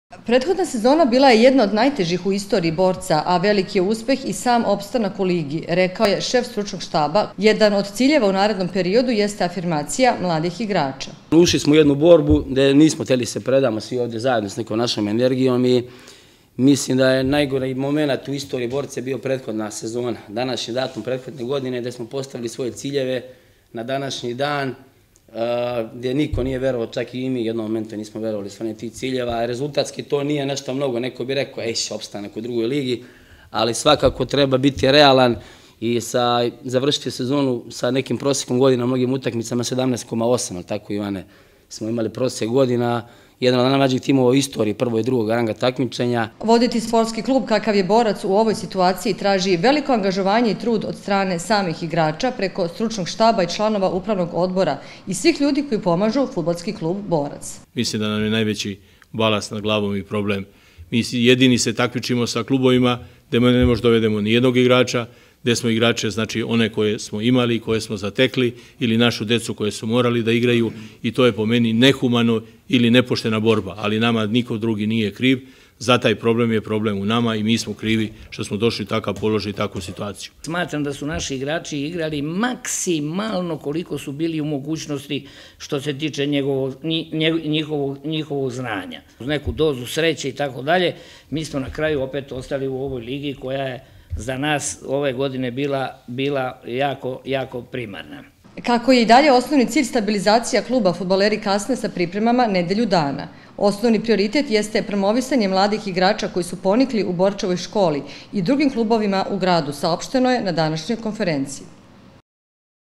08.07.2019. FK BORAC: OSNOVNI CILJEVI SU STABILIZACIJA KLUBA I OPSTANAK U LIGI
Fudbaleri Borca kasne sa početkom priprema zbog kritične finansijske situacije u kojoj se klub nalazi vec dve godine. Stabilizacija kluba i opstanak u ligi osnovni su ciljevi u narednom periodu.

STUDIO ČAČAK

Adresa: Skadarska 7/3, 32000 Čačak

STUDIO UŽICE